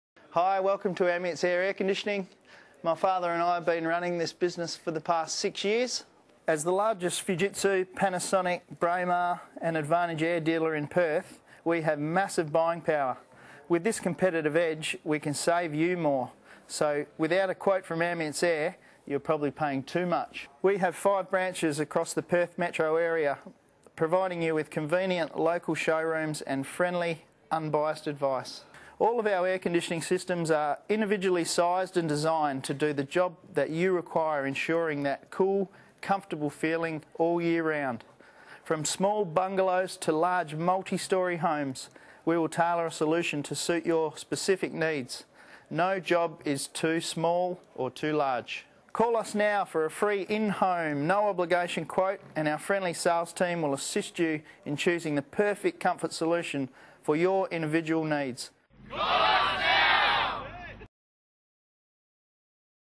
Ambience Air - Perth’s multi-award winning Air-Conditioning & Heating Specialist
Know more about Ambience Air’s history to find out how the company grew to become Perth’s multi-award winning air conditioning and heating specialist. Contact our friendly sales team for a free, no obligation in-home quote and we will assist you in choosing the perfect comfort solution for your individual needs.